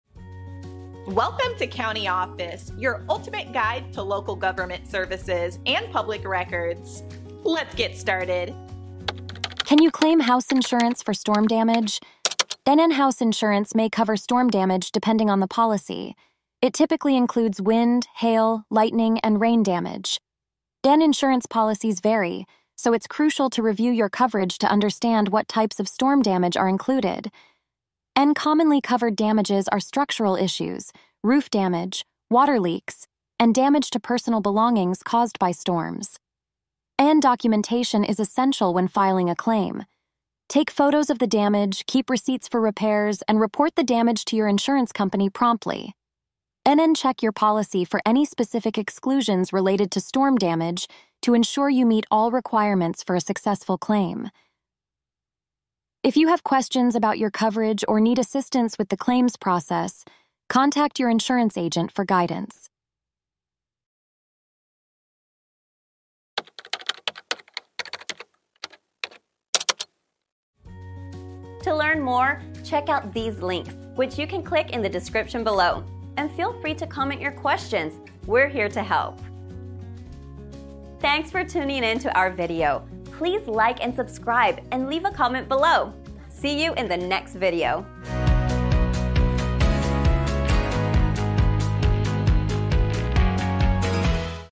Can You Claim House Insurance for Storm Damage? - CountyOffice.org
Can You Claim House Insurance for Storm Damage? In this insightful video, we delve into the topic of claiming house insurance for storm damage. Understanding what types of storm damage are covered by your policy is crucial, as insurance coverage can vary significantly. From wind and hail damage to water leaks and structural issues, knowing what your policy includes is essential for a successful claim.

When it comes to filing a claim for storm damage, documentation is key. Make sure to document the damage with photos, keep receipts for repairs, and promptly report the damage to your insurance company. Checking your policy for any exclusions related to storm damage is also vital to ensure your claim meets all requirements.

If you find yourself with questions about your coverage or need assistance navigating the claims process, don't hesitate to reach out to your insurance agent for guidance. Understanding the ins and outs of your policy can make a significant difference when it comes to handling storm damage claims effectively.

He